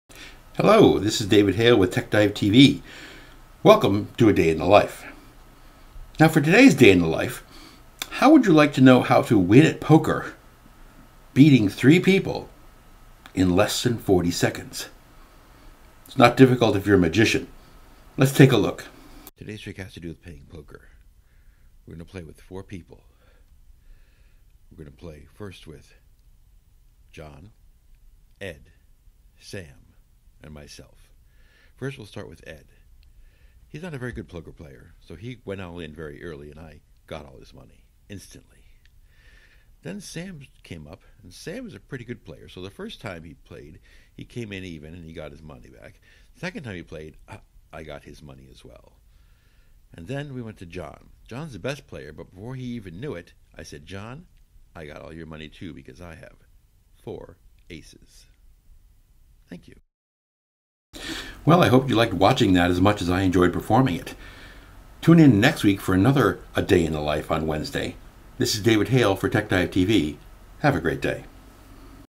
A Day in the Life Episode 12
How would you like to win a poker game in less than 40 seconds?  It is effortless for a magician.